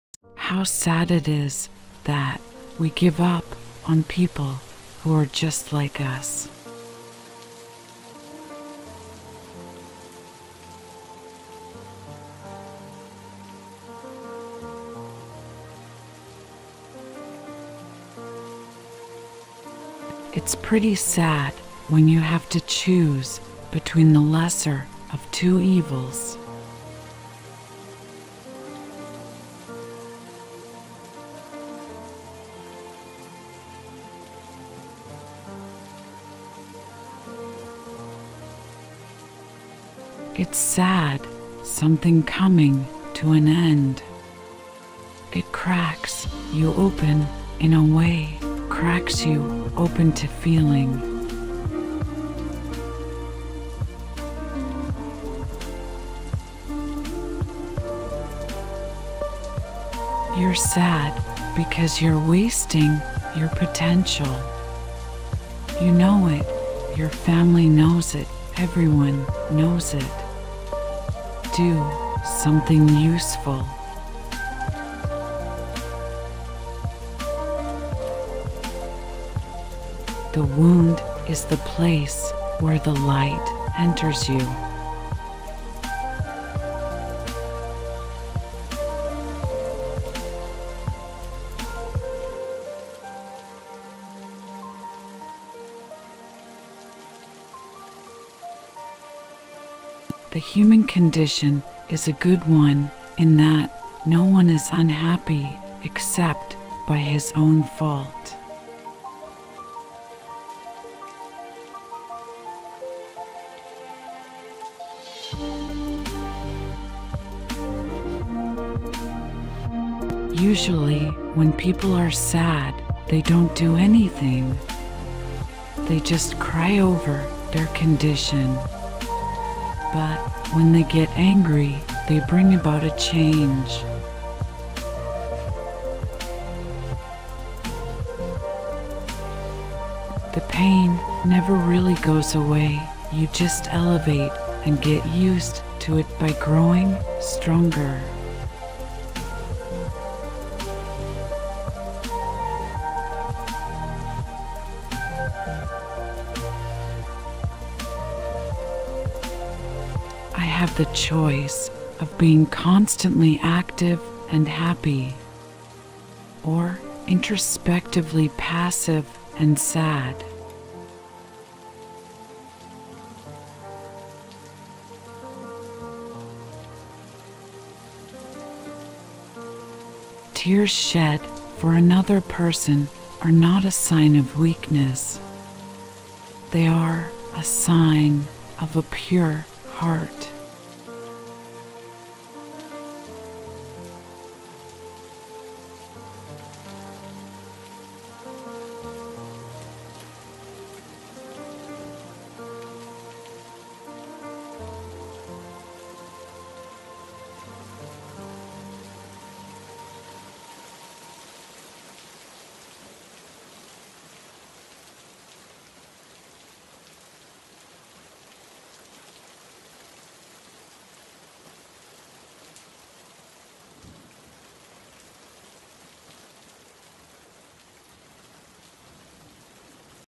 SADNESS QUOTES Discover The Real Sadness Quotes #5 - Eleanor Quinn
SADNESS QUOTES Discover The Real Sadness Quotes

Eleanor Quinn
Words are pale shadows of forgotten names. As names have power, words have power. Words can light fires in the minds of men. Words can wring tears from the hardest hearts.